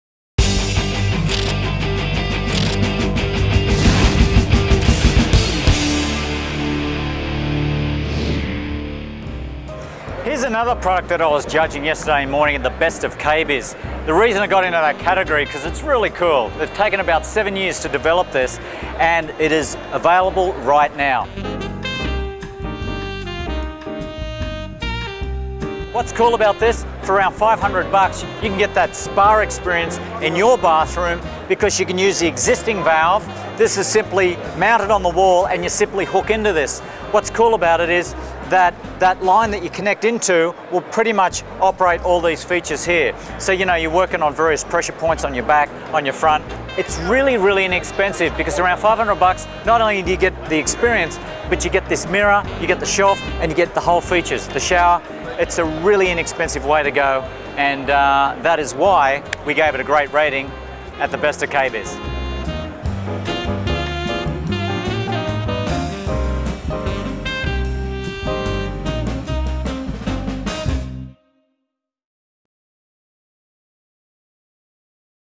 Jet Pro Shower Spas at KBIS
As one of the judges at this year's Kitchen and Bath Show in Vegas, Karl Champley saw many new products, and some stood out more than others. Jet Pro Shower Spas is an affordable shower spa that can be easily installed in minutes, has multiple jets to soothe muscle tension, and is stylish too! Take a look!